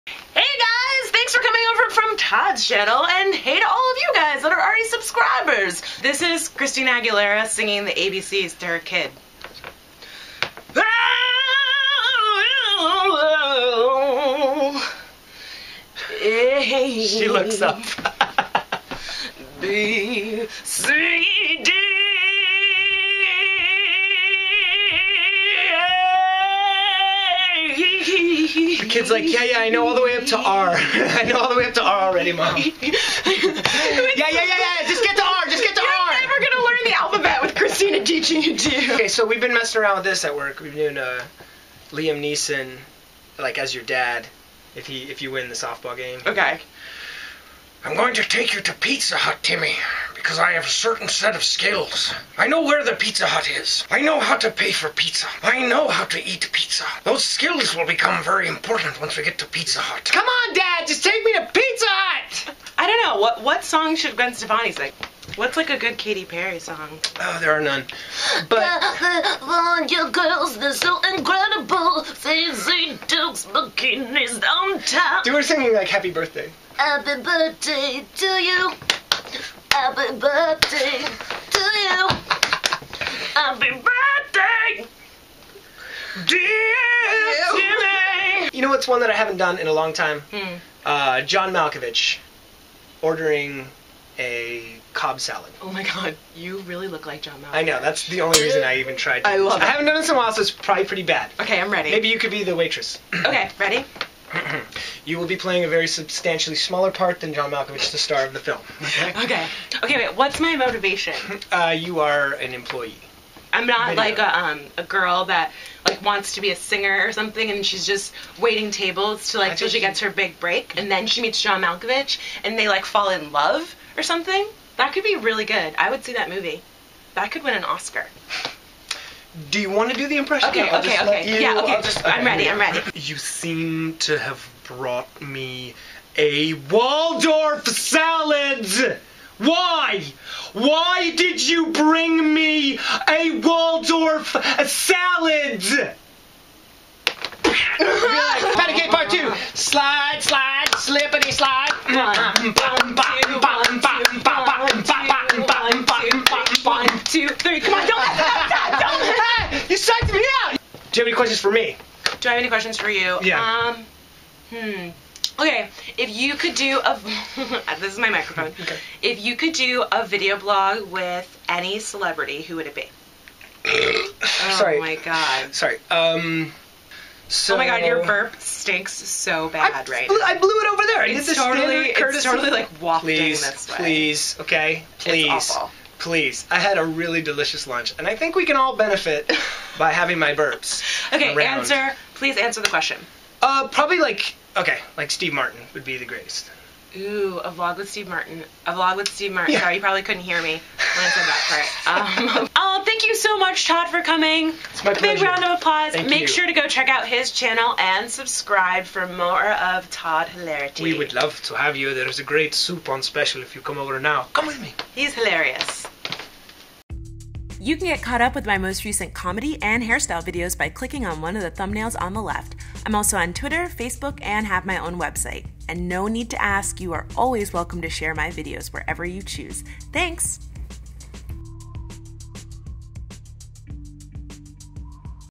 IMPRESSIONS VLOG: Chescaleigh vs Wombatron
Todd (from Barely Political & Key of Awesome) and I vlog it up and do some impressions!